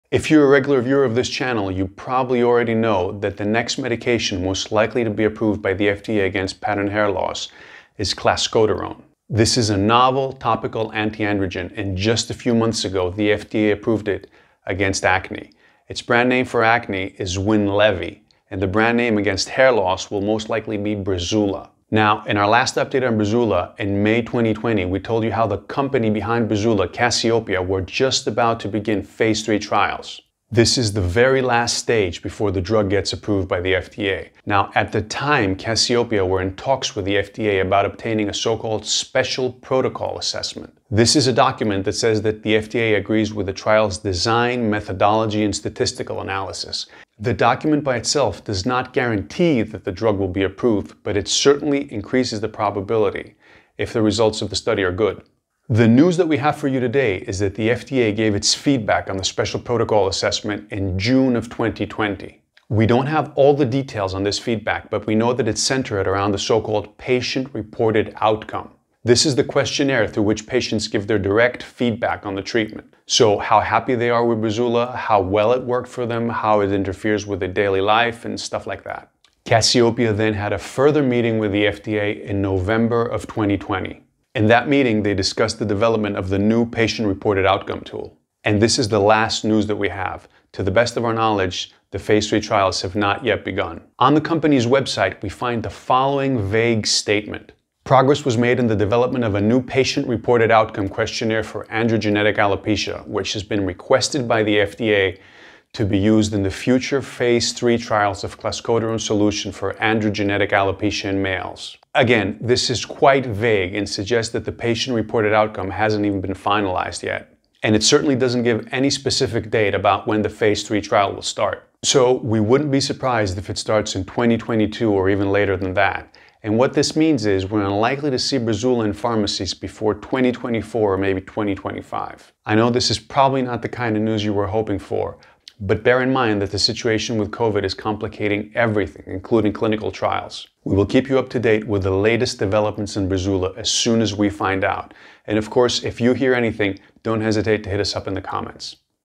What Happened to Breezula (clascoterone)!?
So, if you’re a regular viewer of this channel, you probably know that the next hair loss medication most likely to be approved by the FDA is clascoterone.
This is a novel topical antiandrogen, and just a few months ago the FDA approved it against acne, under the brand name Winlevi.
The version for pattern hair loss will likely be called Breezula.
In our last update on Breezula, In May 2020,
 we told you how Cassiopea, the makers of the drug, were just about to start the final, phase 3 trials on men with hair loss.
This is the very last research stage before full approval.
At that time Cassiopea were in talks with the FDA to get a so-called special protocol assessment.
This means that the FDA agrees with the trial’s design, methodology and statistical analysis.
It doesn’t guarantee by itself that the drug will eventually be approved. But it certainly makes it more likely, depending on the results of the trial.
The news that we have is that the FDA gave its feedback on the Special Protocol Assessment in June of 2020.
We don’t have all the details on this feedback, but we know it involved the Patient Reported Outcome.
This is the questionnaire through which patients give their direct feedback on the treatment.
So how happy they are with Breezula, how well it’s working, how it interfered with their day to day life - stuff like that.
Cassiopea then had a further meeting with the FDA in November 2020. In that meeting they discussed the development of the new patient Patient Reported Outcome tool.
That is the last news we had. To the best of our knowledge, the phase 3 trials have still not started.
On the company’s website we find the following vague statement:

Progress was made in the development of a new Patient Reported Outcome (PRO) Questionnaire for AGA which has been requested by the FDA to be used in the future Phase III trials of clascoterone solution for AGA in males.

Again, this is quite vague, and suggests that the PRO hasn’t even been finalized yet.
And it certainly doesn’t give any specific date about when the Phase III trial will start.
So we wouldn’t be surprised if it starts in 2022, or even later.
Meaning we’re not likely to see Breezula in pharmacies before 2024 or even 2025.
Probably not the kind of news you were hoping for. But bear in mind everything nowadays is complicated by the COVID situation, including clinical trials.
We will keep you up to date with the latest developments on Breezula as soon as we find out.
And if you do hear anything yourself, don’t hesitate to hit us up in the comments.